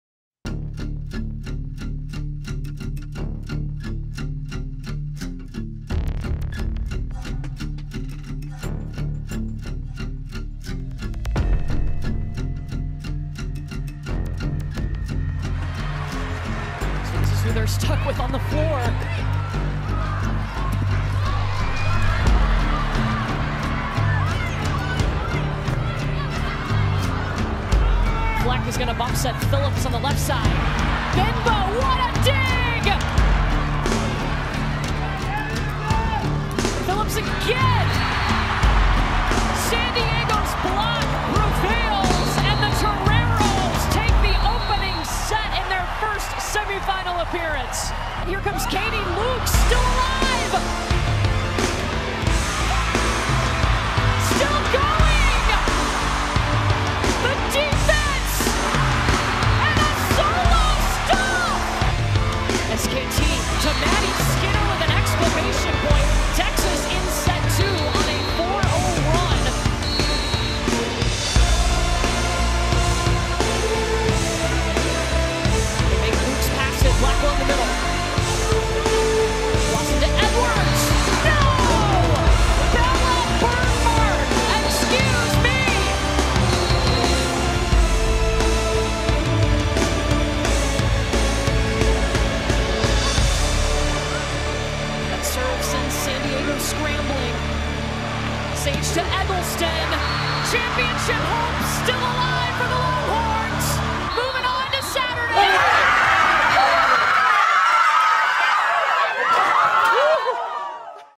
Cinematic highlights: Texas vs. San Diego — NCAA volleyball semifinals
Catch a courtside look at Texas volleyball's win over San Diego in this cinematic recap of the 2022 NCAA women's volleyball national semifinals.